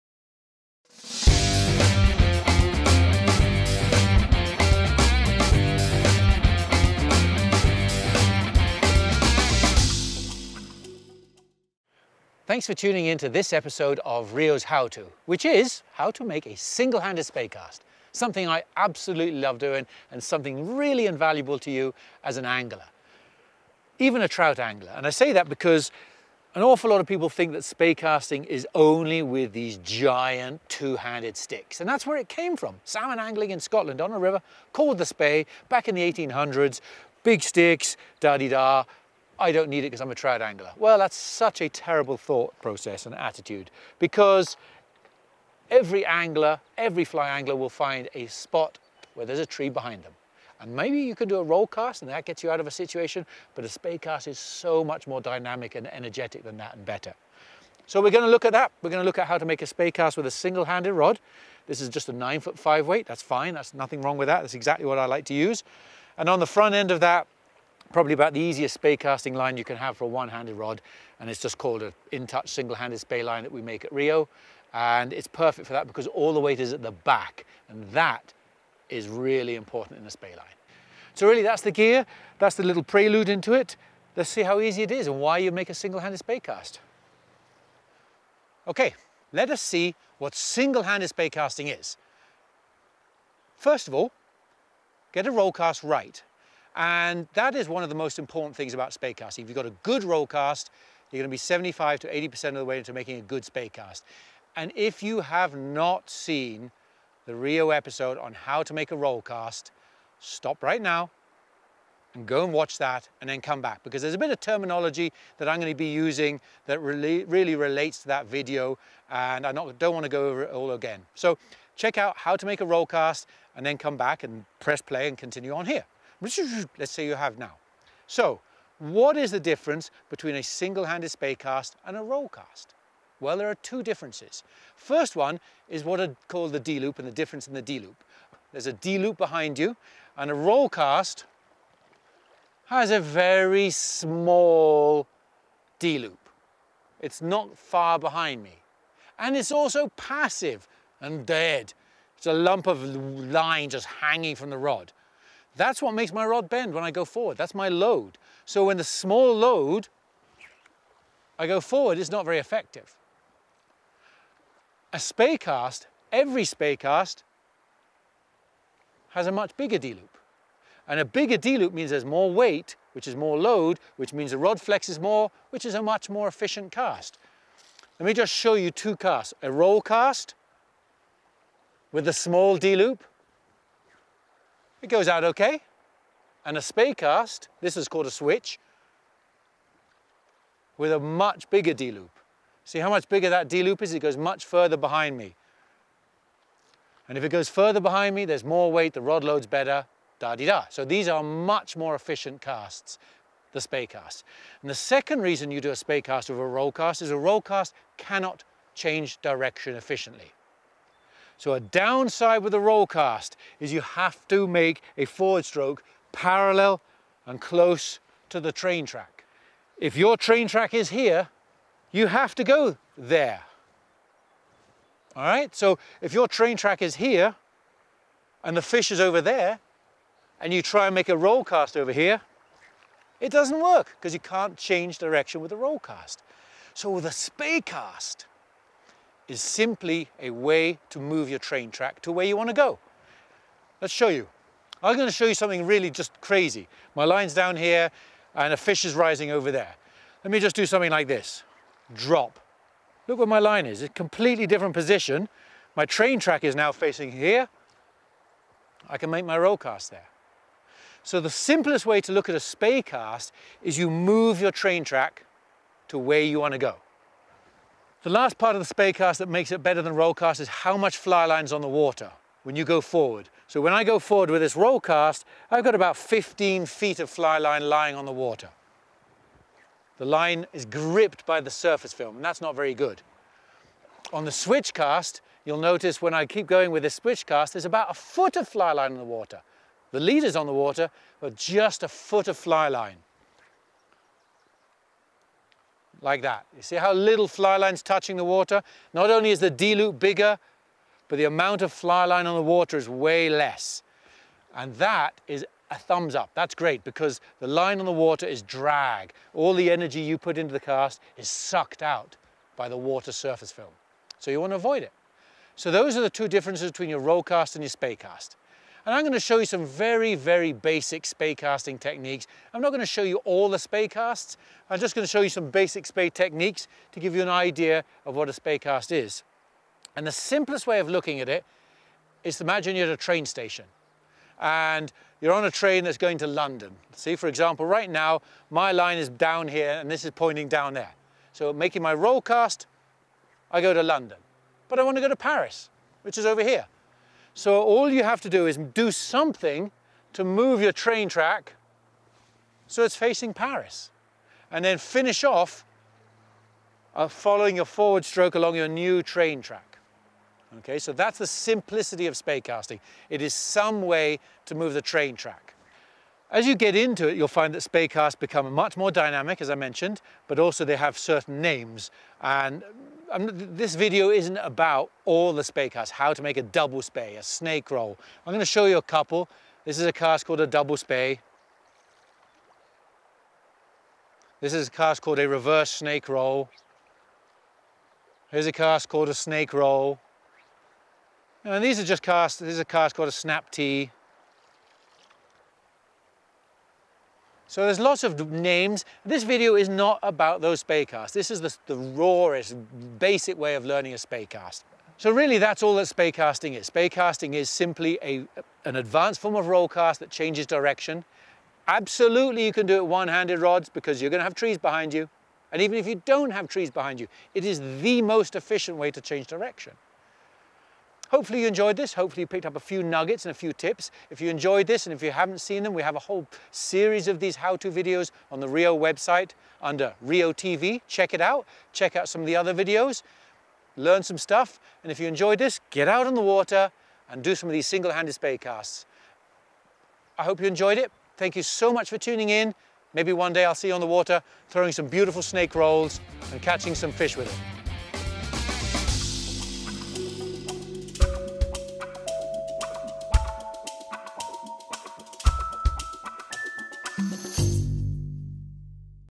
Single Handed Spey Cast | How To
Simon Gawesworth explains and demonstrates the basics of Spey casting with a single-handed rod. This video doesn't delve into any particular Spey cast, rather it shows the concept of how to make a Spey cast with a single-handed rod, and lays a very solid foundation for improving casting skills. Many fly fishers believe that Spey casting is a tool only for salmon and steelhead anglers, and for using with long, two-handed rods, but Simon shows how important it is for trout anglers to have a knowledge of the Spey cast in their armory. In addition, Simon explains how this knowledge can help a caster make more efficient casts with obstructions behind, make angle changes with ease, and introduces the evolution a caster should go through form the humble roll cast, to the powerful single-handed Spey casts.